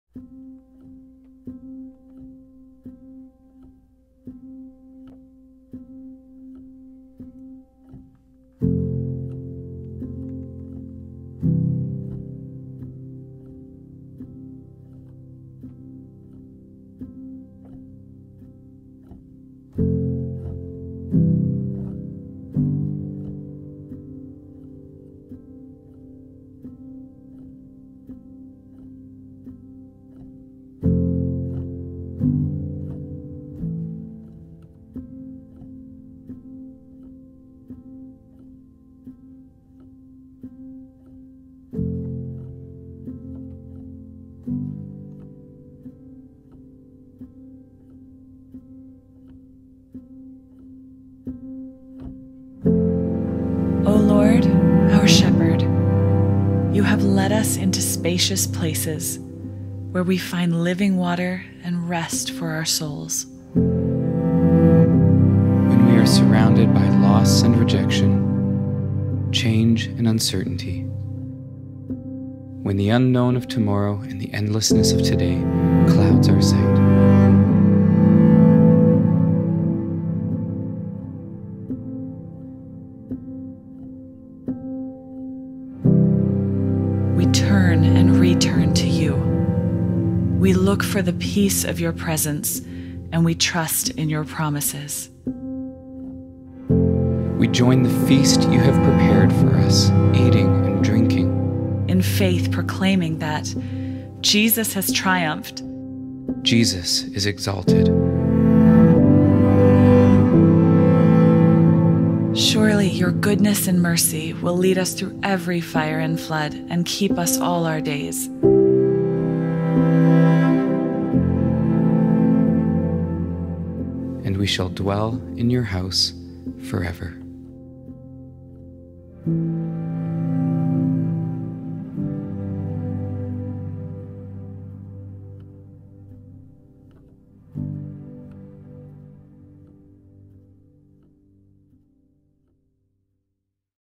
Midweek Worship: Psalm 23 Meditation
"O Lord, our Shepherd, you have led us into spacious places, where we find living water and rest for our souls."

We offer you this visual meditation, not as a piece of content to keep up with, but as an invitation for you to take a moment to be still.  Give yourself the gift of a moment to breathe.  And when the visual meditation is done, allow yourself to linger with it, listening, before returning to all that this day has for you.